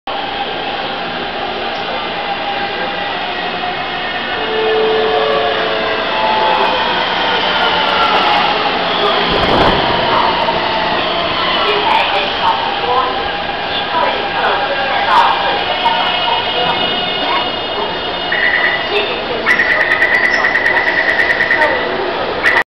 台北捷運C321型進站(忠孝敦化).avi
台北捷運板南線C321型進站(忠孝敦化),攝於2012年1月14日,去公館玩回來,等了一下想說拍彩繪車,結果這班車來的時候我下一跳!它駕駛室上方的路線顏色顯示沒亮!?好奇怪~